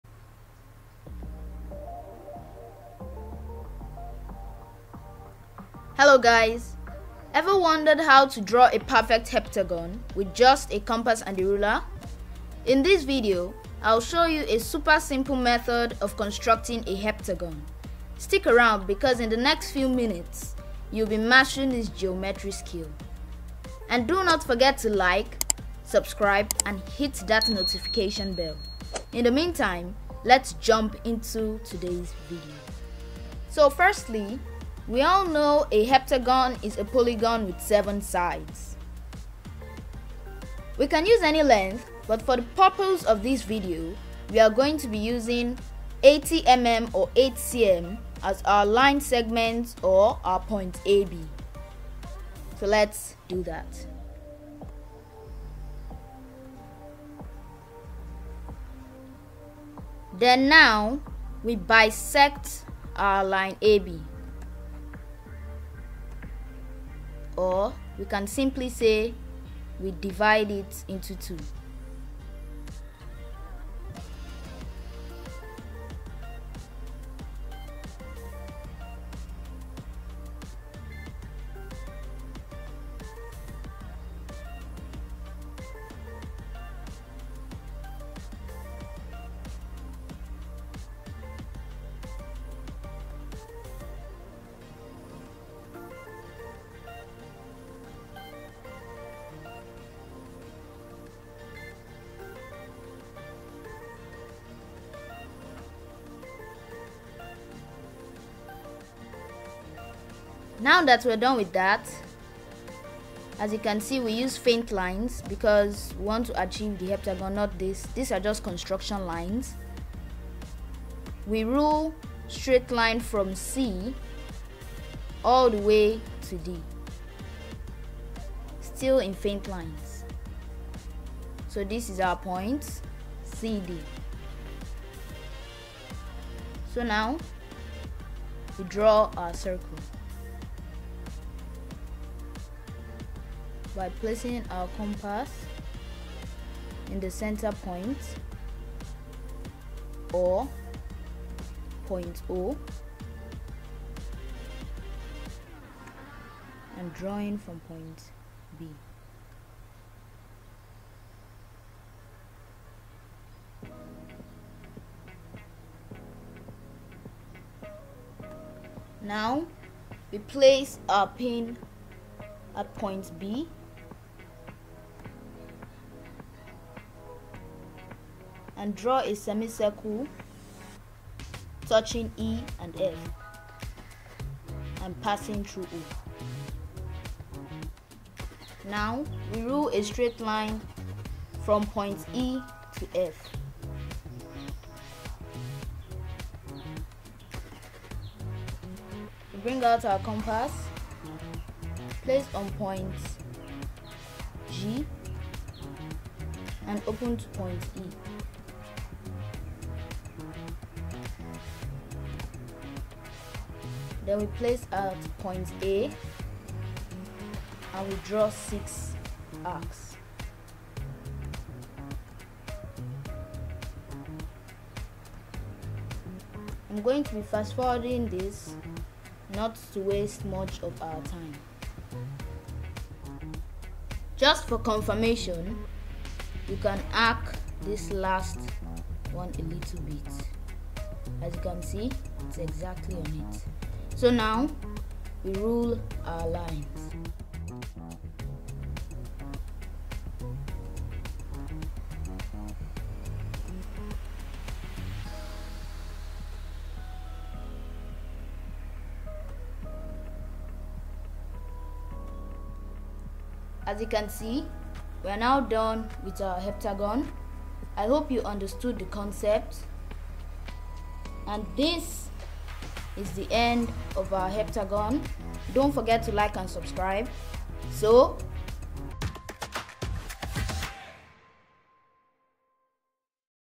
How to construct a Heptagon
In this video, we will be learning how to construct a heptagon using just a compass and straightedge.

Follow along as we guide you through drawing a circle, dividing it into seven equal parts, and connecting the points to form a perfect seven-sided polygon. Impress your friends and boost your geometry skills with this fun and educational tutorial!